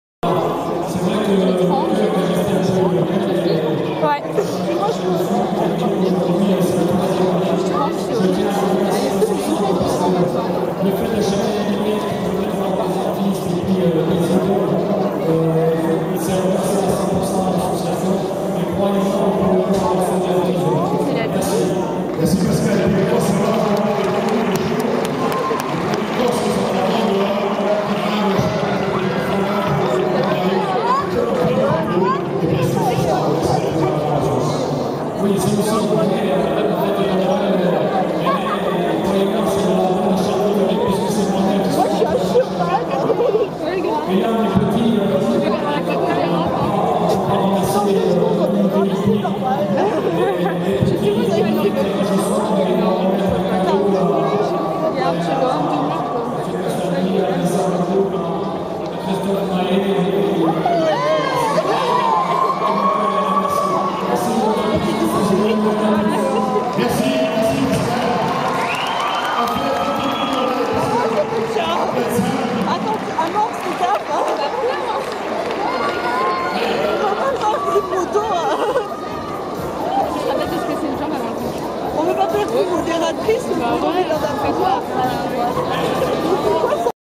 [Multi-Artiste] Beach Soccer @ Palais des Sports
Beach Soccer, au Palais des Sports de Lyon, prise de photos officielles //
Le Samedi 19 Décembre 2009